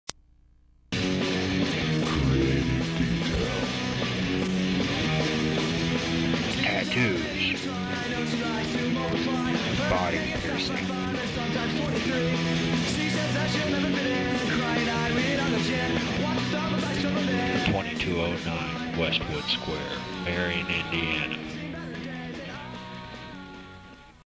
2004 Commercial salvaged from a VHS tape
It was around 2004, I was using MiniDV tapes and Adobe Premiere at the time. I made VHS copies to pass out for promotional use.
Creative Detail R.I.P.
Now known as II The Soul Tattoo & Body Piercing